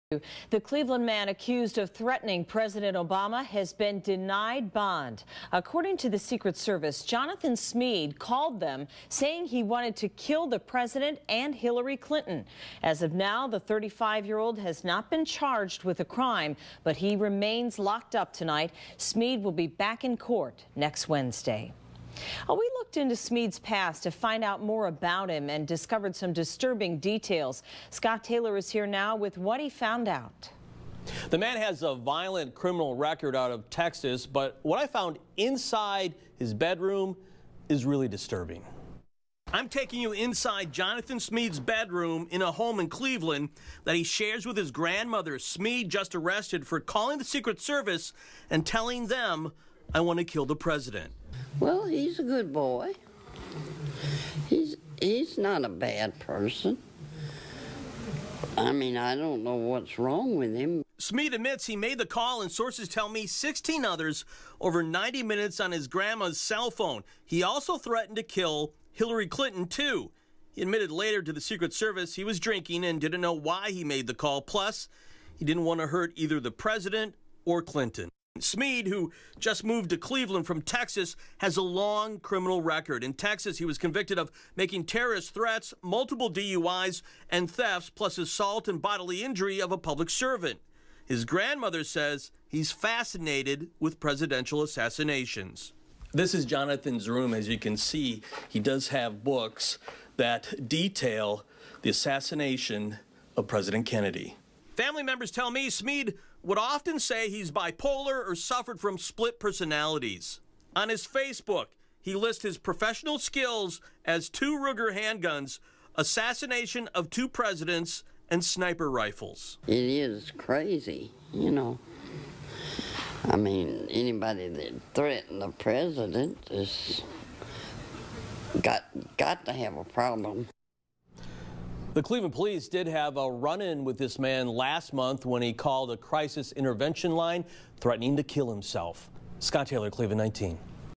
EXCLUSIVE: GO INSIDE THE BEDROOM OF THE CLEVELAND MAN WHO THREATED THE PRESIDENT
Jonathan Smead threatened to kill President Obama.  Scott Taylor talks to his grandmother and shows you what he found inside Smead's bedroom.